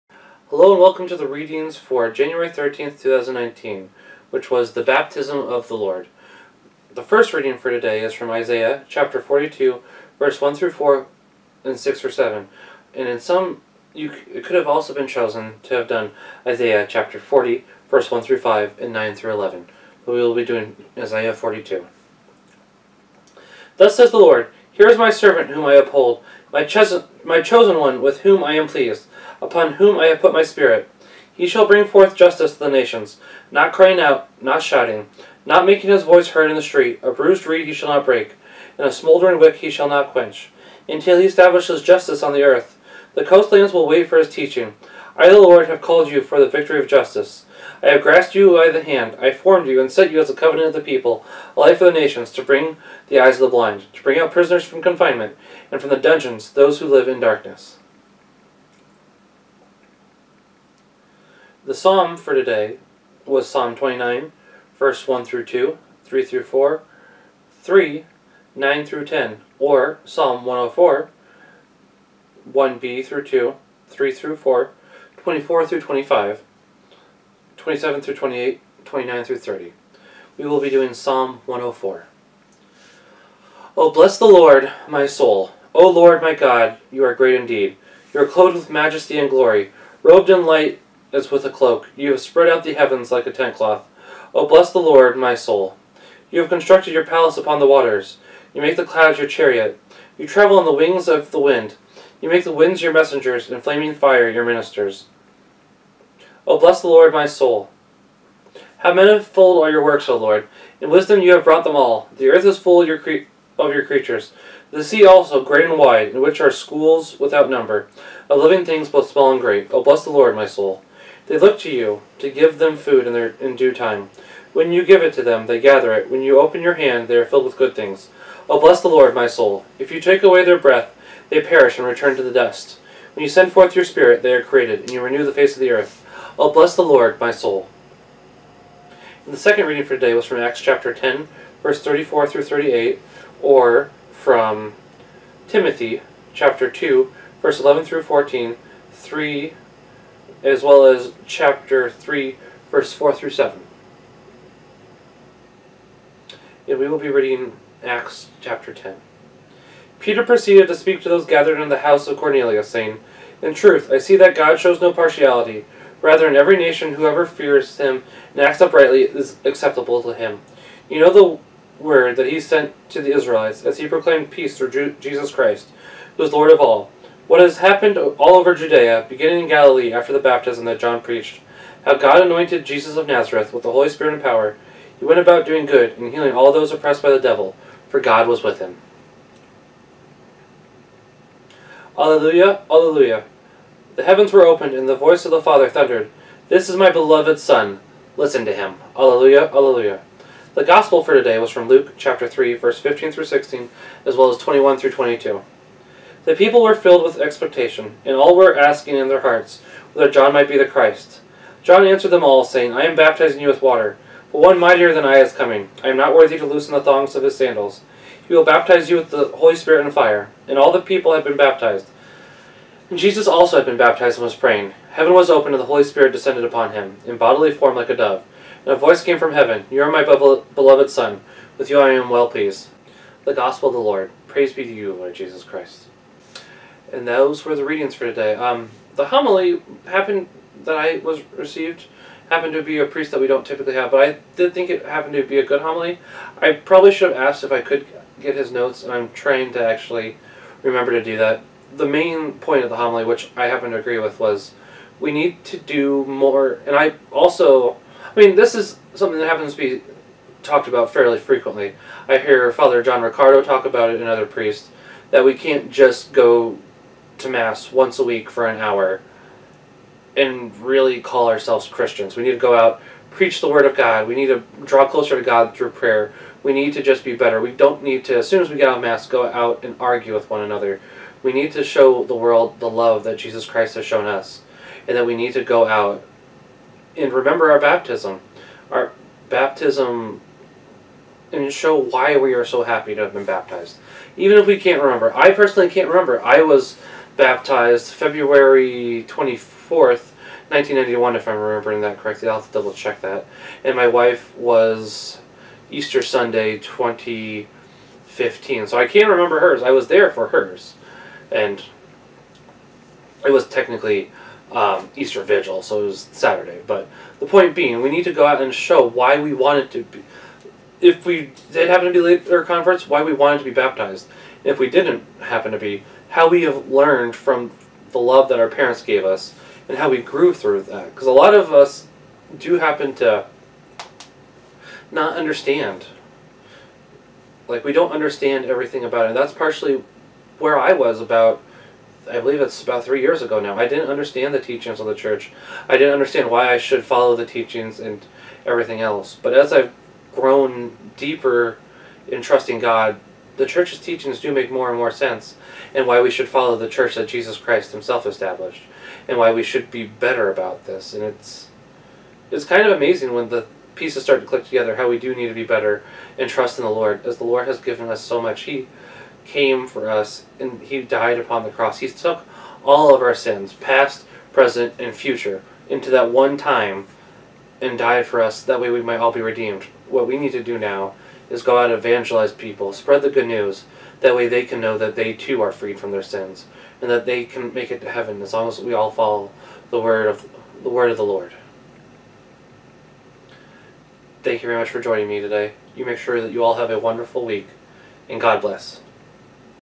Readings for the Baptism of the Lord - 1/13/2019
This is the First, Second and Gospel reading for The Baptism of the Lord - 1/13/19